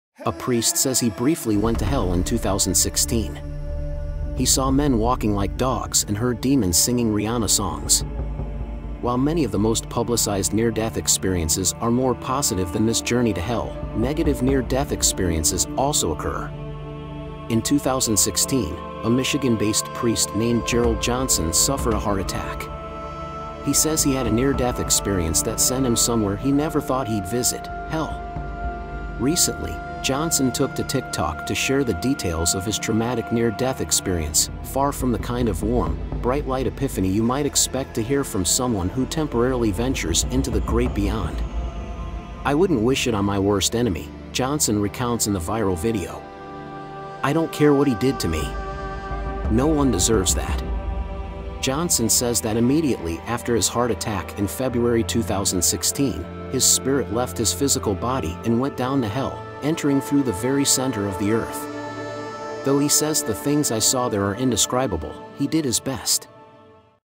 A Guy Says He Had a Heart Attack and Went to Hell in 2016. What did he see?
A priest says he briefly went to hell in 2016.
He saw men walking like dogs and heard demons singing Rihanna songs.
While many of the most publicized near-death experiences are more positive than this journey to hell, negative NDEs also occur.
In 2016, a Michigan-based priest named Gerald Johnson suffered a heart attack. He says he had a near-death experience (NDE) that sent him somewhere he never thought he’d visit: hell.

Recently, Johnson took to TikTok to share the details of his traumatic NDE—far from the kind of warm, bright-light epiphany you might expect to hear from someone who temporarily ventures into the great beyond.

“I wouldn’t wish it on my worst enemy,” Johnson recounts in the viral video. “I don’t care what he did to me. No one deserves that.”

Johnson says that immediately after his heart attack in February 2016, his spirit left his physical body and went down to hell, entering through “the very center of the Earth.” Though he says “the things I saw there are indescribable,” he did his best.